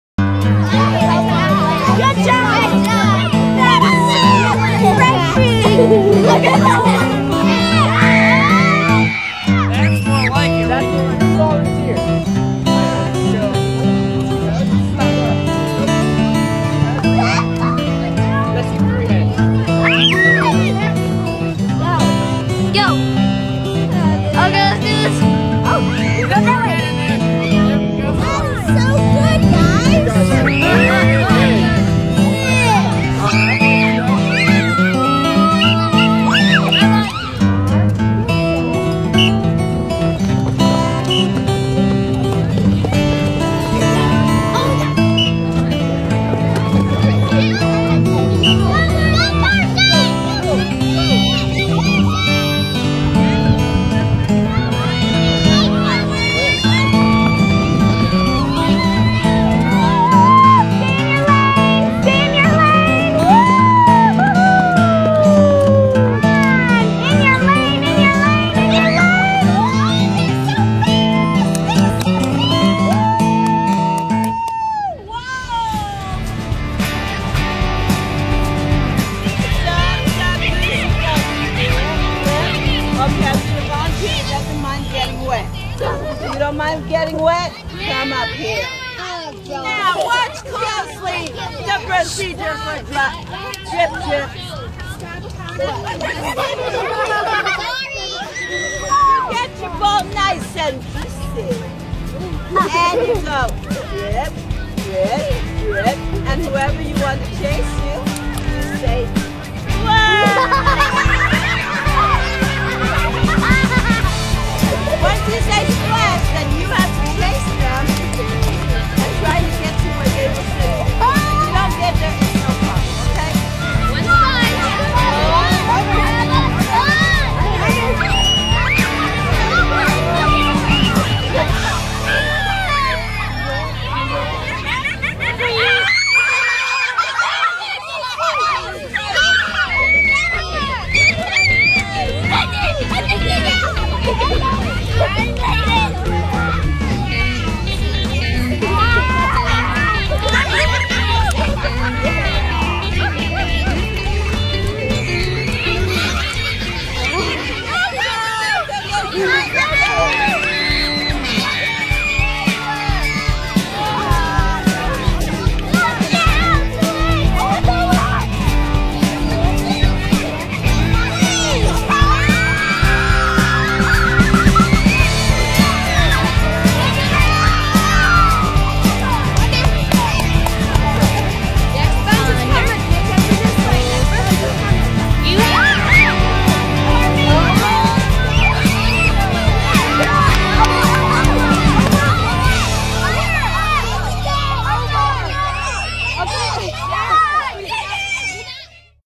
Heritage Christian School Elementary Field Day 2017
What a wonderful elementary field day. The weather was great (between the storms). The kids had so much fun!